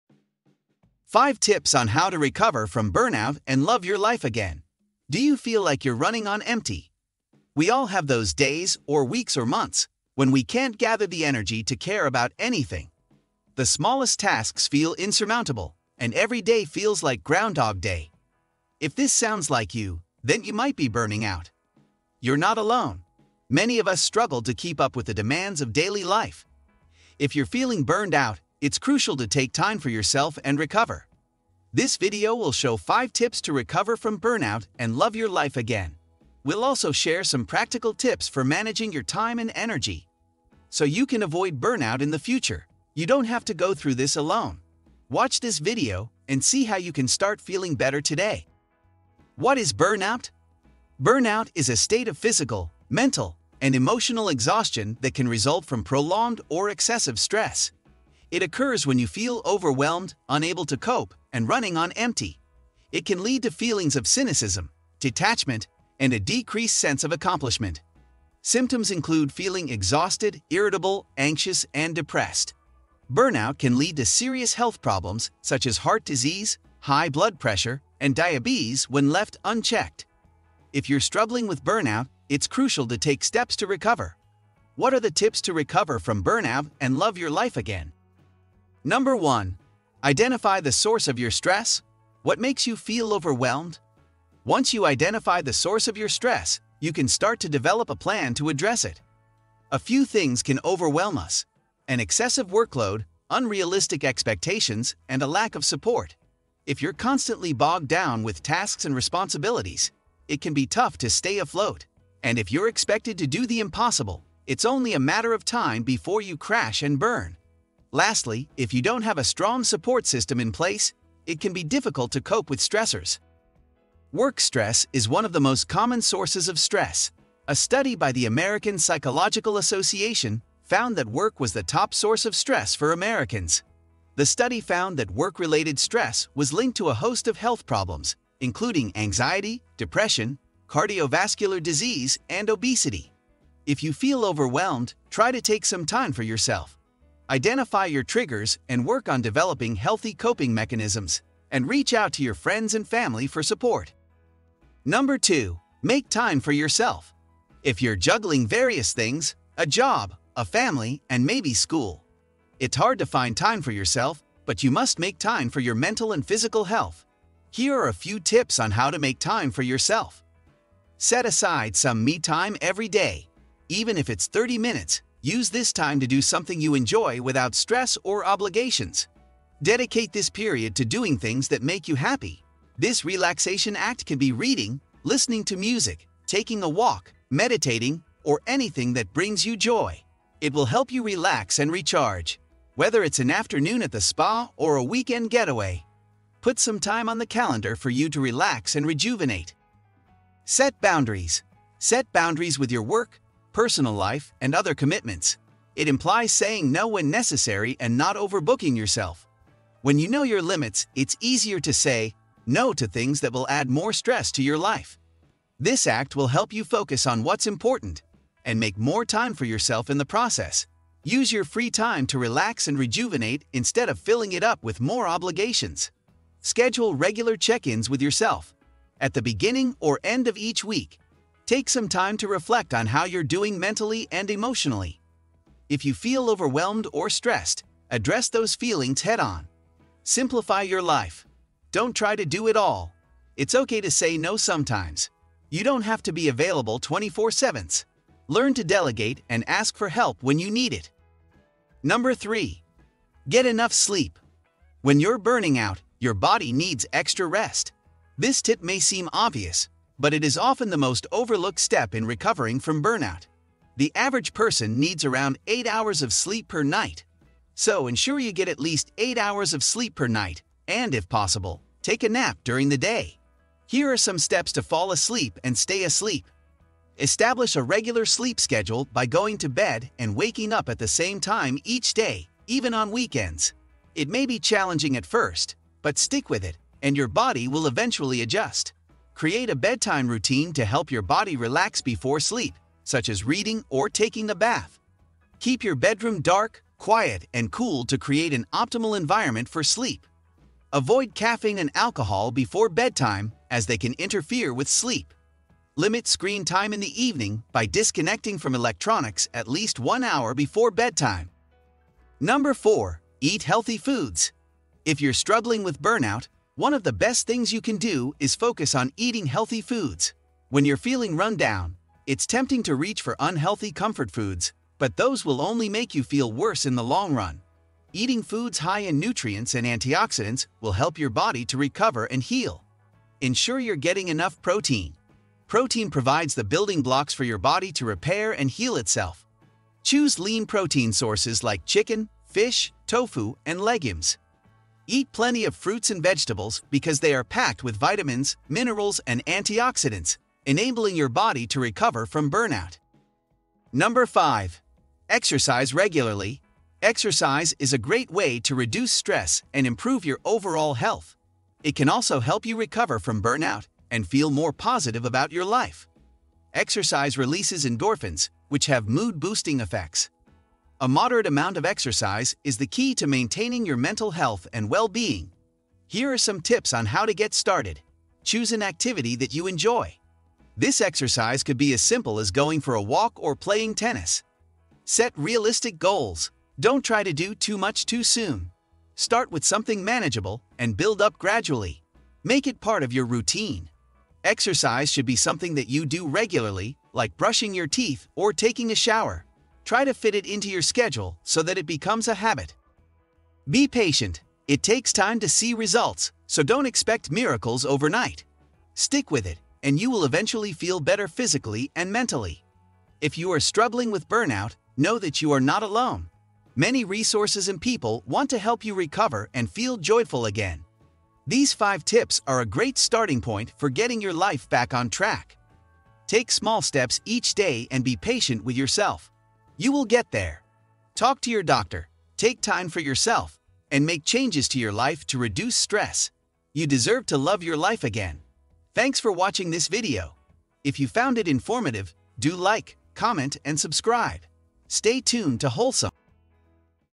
Recover from burnout and love your life again
Do you feel like you're running on empty? *This video will show five tips to recover from burnout and love your life again.* We'll also share some practical tips for managing your time and energy.

Check out our video on 5 signs you're burning out and what you can do about it

Chapters

0:00 Intro
0:49 What is burnout?
1:29 Identify the source of your stress
2:35 Make time for yourself
4:13 Get enough sleep
5:13 Eat healthy foods
5:55 Exercise regularly

Summary

What is burnout?
Burnout is a state of physical, mental, and emotional exhaustion that can result from prolonged or excessive stress.

What are the tips to recover from burnout and love your life again?

Identify the source of your stress
What makes you feel overwhelmed? Once you identify the source of your stress, you can start to develop a plan to address it.

Make time for yourself
If you’re juggling various things: a job, a family, and maybe school. It’s hard to find time for yourself, but you must make time for your mental and physical health.

Get enough sleep
When you're burning out, your body needs extra rest. This tip may seem obvious, but it is often the most overlooked step in recovering from burnout.

Eat healthy foods
If you're struggling with burnout, one of the best things you can do is focus on eating healthy foods. When you're feeling run down, it's tempting to reach for unhealthy comfort foods, but those will only make you feel worse in the long run.

Exercise regularly
Exercise is a great way to reduce stress and improve your overall health. It can also help you recover from burnout and feel more positive about your life.

References